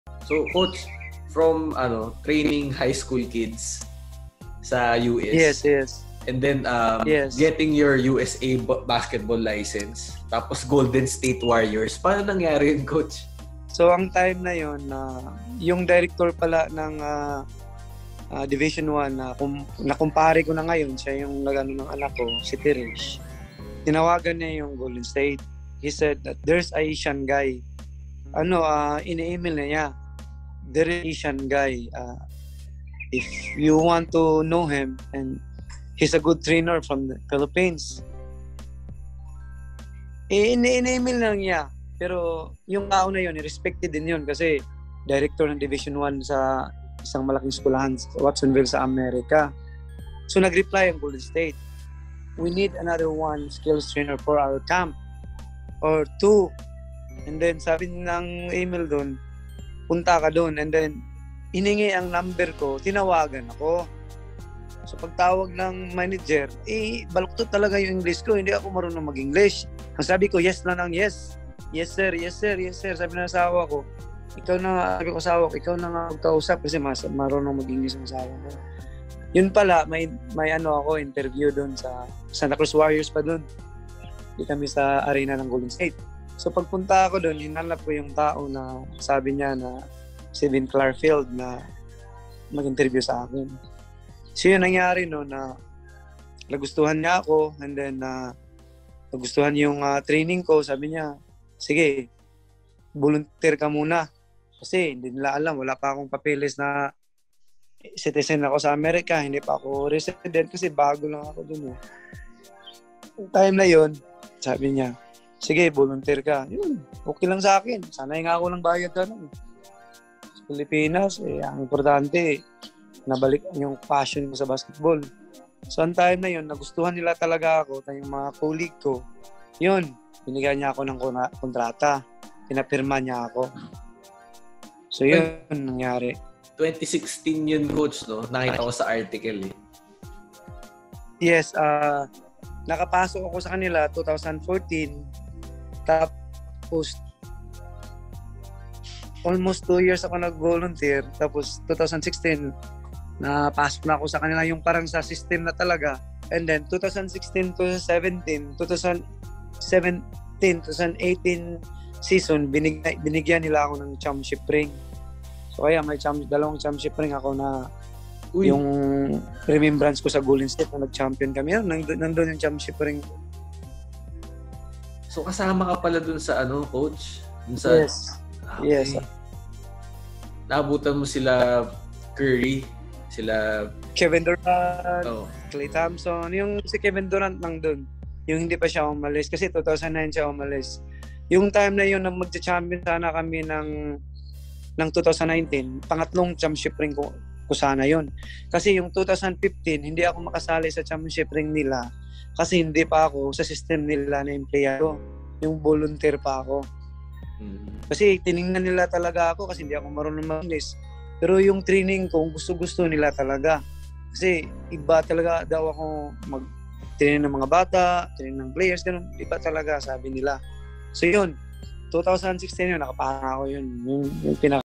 "JEEPNEY BARKER to NBA CHAMPION with the Golden State Warriors" | Coach Jeff Codera | STEPBACK
Sa "Stepback" series, pinag usapan namin ni Coach Jefferson Codera (Filipino NBA Trainer) ang istorya kung paano siya napasama sa Golden State Warriors. Siya ay isang 2 Time NBA Champion, nabigyan siya ng dalawang championship ring dahil sa mga kontribusyon niya sa Warriors.

Si Coach Jeff "Man of a Million Drills" Codera ay isang Filipino trainer based sa Cebu at sa Watsonville, California.